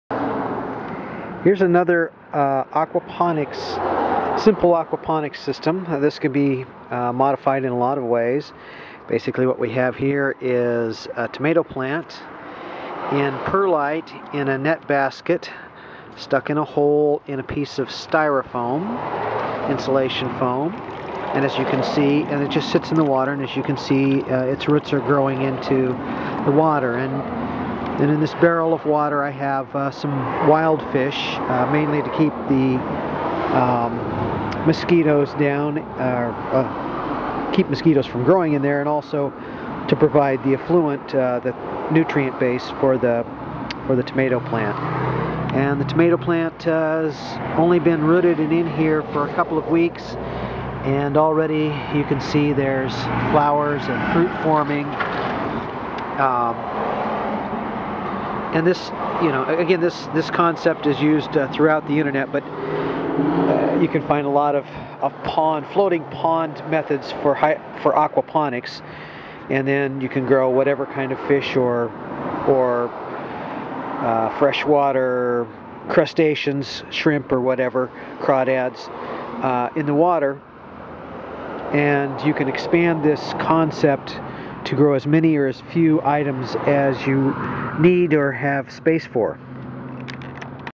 simple floating aquaponics system
A very simple flotation type aquaponic system.  Very simple and scalable way to grow fresh water fish or crustaceans for food in water and using the effluent from the fish to feed the plants.